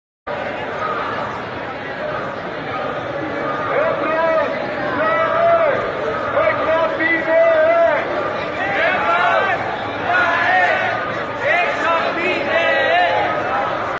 Atina'da göçmenlere destek yürüyüşü: Birlikte yaşayabiliriz
Yunanistan'ın başkenti Atina'da binlerce kişi, sınırların göçmenlere açılması için destek yürüyüşü yaptı.